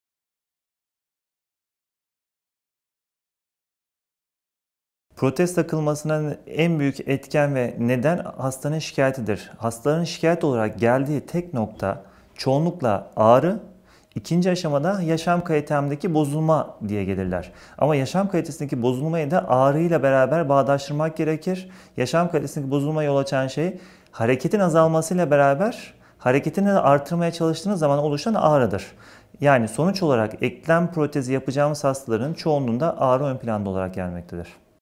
Ağrının derecesi protez takılmasına sebep olur mu?
Acıbadem Maslak Hastanesi, Ortopedi ve Travmatoloji Uzmanı Dr. Metin Uzun, Protez üzerine soruları yanıtlıyor;